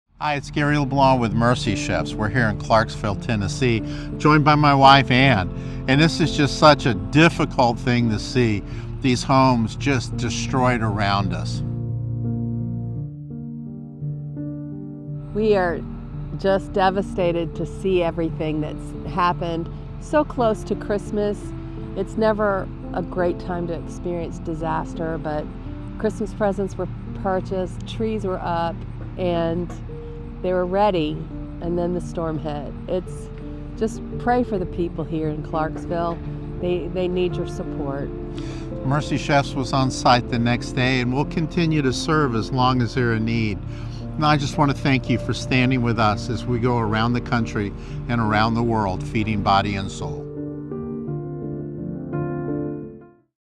Tennessee Tornado Relief Update
Just a few days ago, the people of Clarksville were ready for Christmas. Their homes were decorated and presents were wrapped under the tree. But then the storm hit, and their lives were changed forever.

CoFounders Gary and Ann LeBlanc share an emotional update from the ground in Tennessee where Mercy Chefs is already serving hot meals to those affected by the deadly tornado.